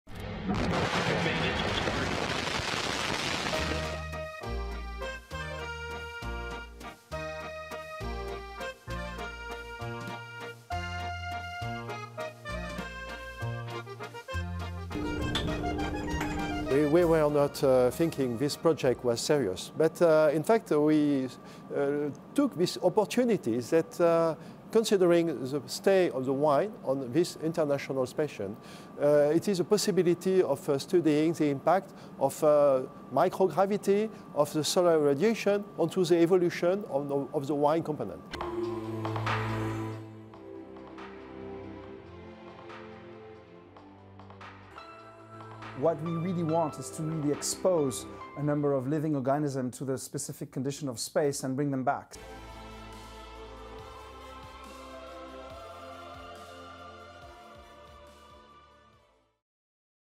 Viskiden sonra şarap da iklim krizi araştırmaları kapsamında uzaya çıktı
Fransa’nın dünyaca ünlü Bordeaux şarabı, iklim krizi ve biyolojik araştırma kapsamında özel koruyucu kutuları içinde Uluslararası Uzay İstasyonu’na (ISS) gönderildi. …